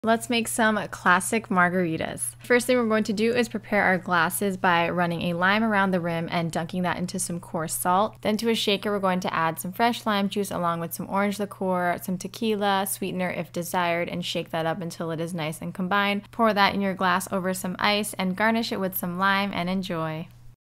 Classic Margaritas 😍
We use 1 oz. each lime and Cointreau, 1.5 oz tequila and a splash of agave :)